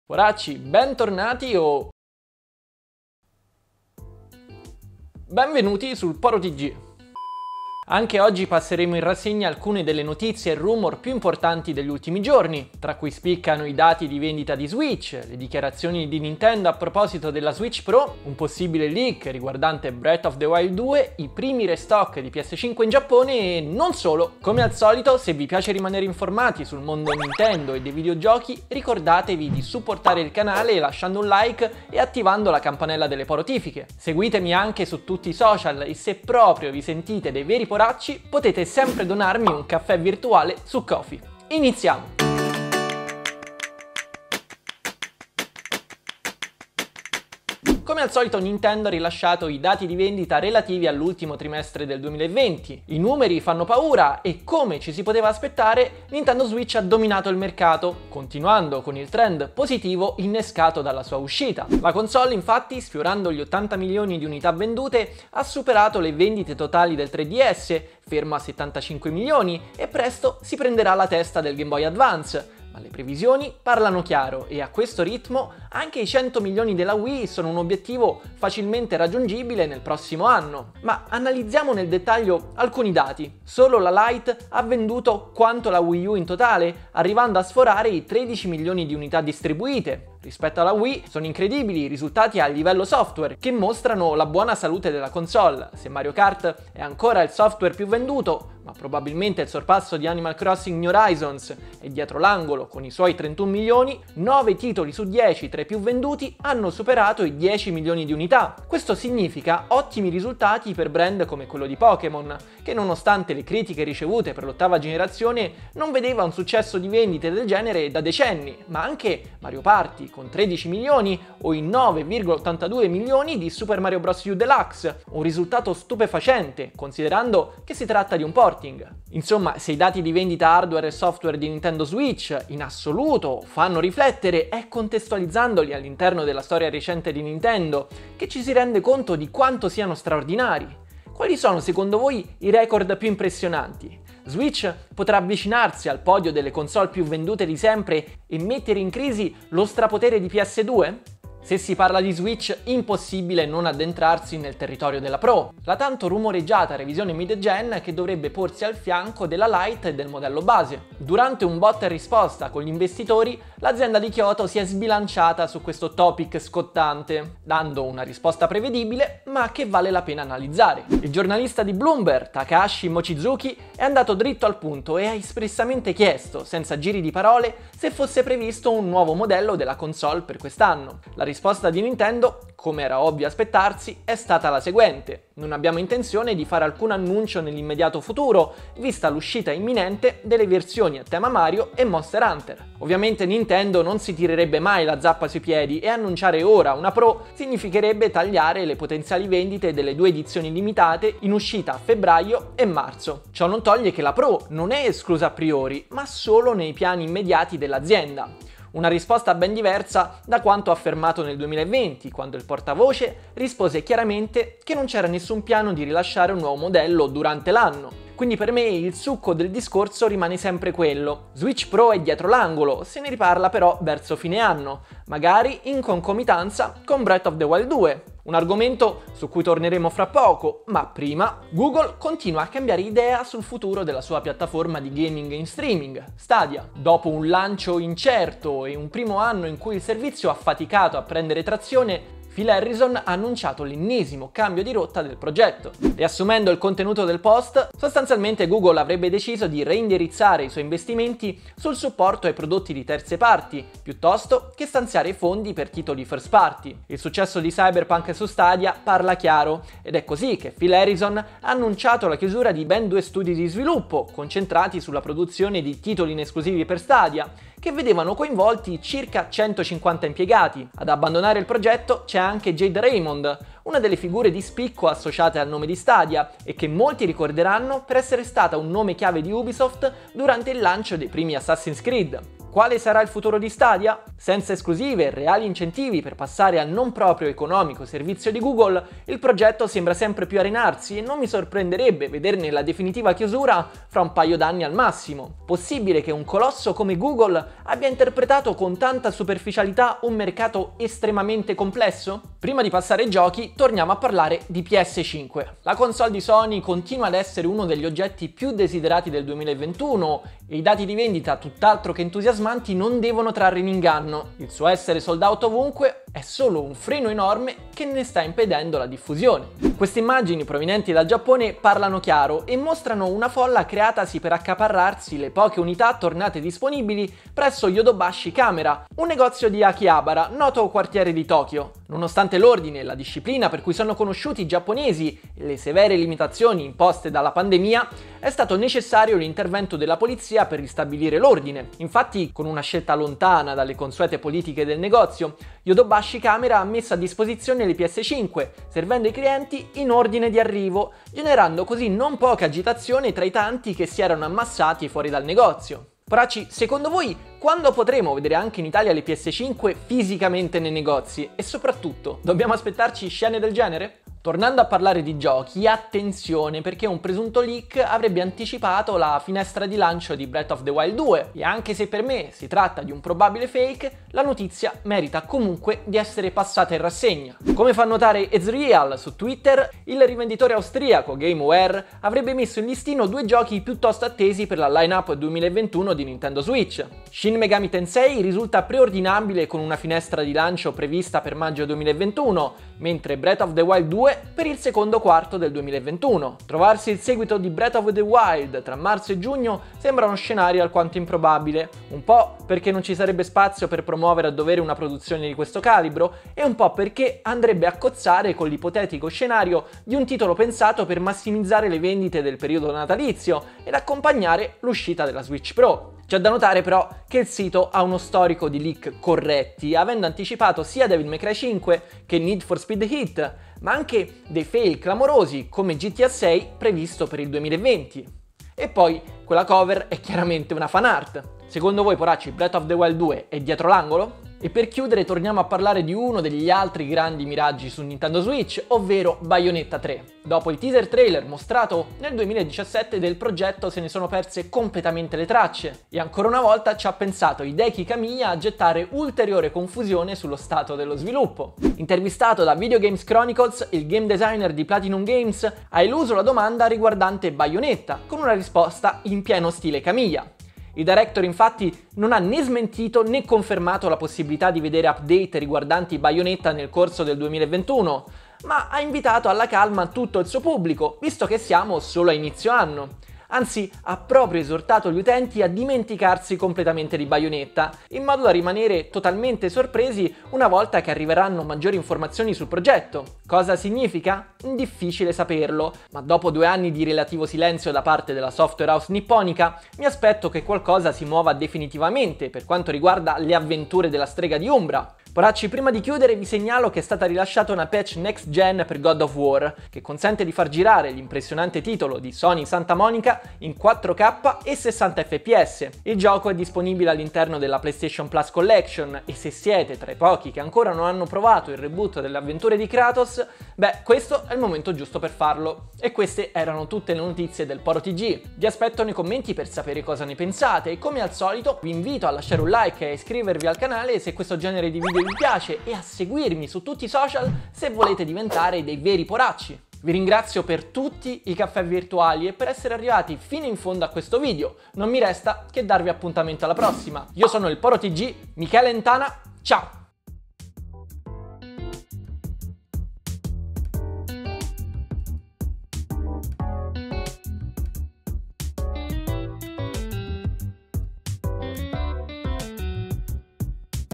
Vendite RECORD per SWITCH, PS5 torna disponibile ma... Un LEAK anticipa BOTW2? | TG PORO
Nintendo Switch continua a infrangere diversi record di vendita. Con oltre 79 milioni di console vendute ha superato le vendite totali del 3DS e si appresta a oltrepassare anche il GBA. Riuscirà a battere Nintendo Wii superando i 100 milioni di unità vendute?
Scene di panico in Giappone per un restock di PS5 avvenuto a Tokyo. La console di Sony scatena scene di isteria nei negozi anche in Giappone, confermando le la console Playstation 5 è tra gli oggetti più desiderati del 2021. Quanto toernerà disponibile in italia?
Nintendo si è espressa sul futuro di Switch Pro, rispondendo a delle domande degli investitori. Questa volta però pur negando l'esistenza di una revisione mid-gen la Switch pro non è stata esclusa a priori. Vedremo Switch pro nel 2021 con Botw2?
Un retailer austriaco riporta la data di uscita di The Legend of Zelda Breath of the Wild 2 come prevista per il Q2 del 2021. il seguito di breath of the Wild è dietro l'angolo? Queste e altre notizie nel PORO TG di OGGI! Cosa ne pensate? Io sono il Poro Tg, Michelentana, ciao!